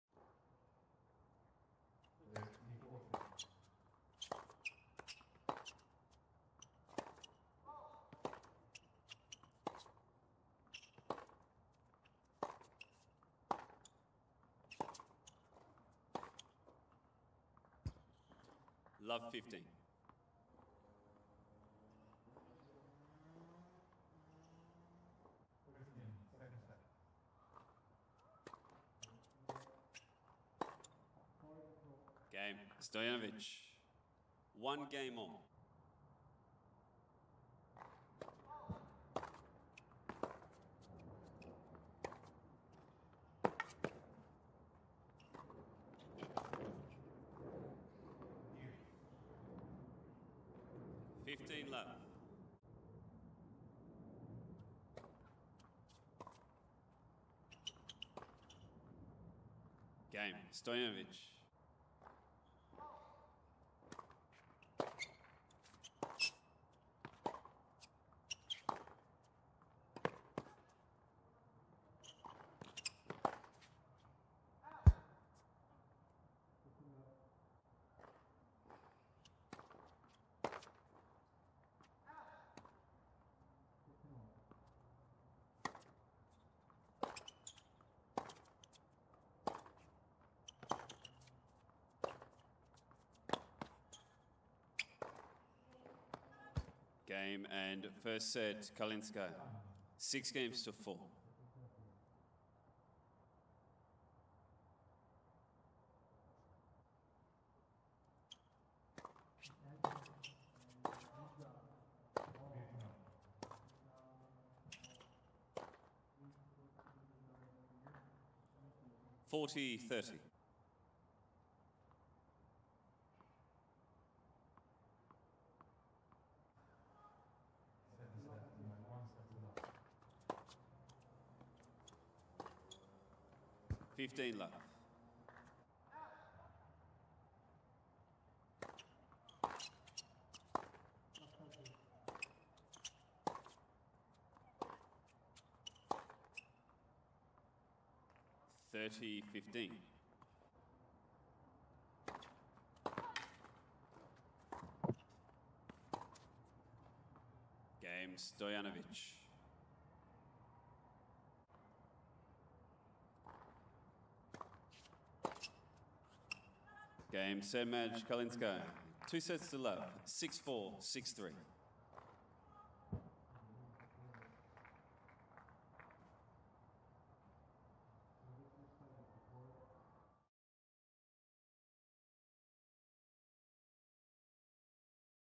Anna Kalinskaya vs Nina Stojanovic | US Open 2020 Round 1
Anna Kalinskaya takes on Nina Stojanovic in Round 1 of the US Open 2020.